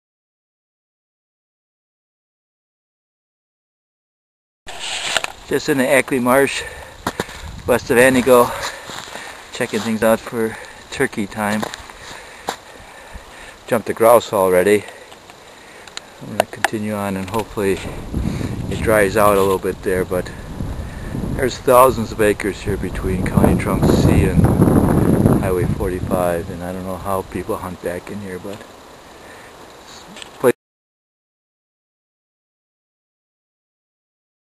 Ackley Wildlife Area on April 16,2020
Short walk for a few hours and looking for turkey signs and there were plenty in areas. A bit wet but next time will be knee boots.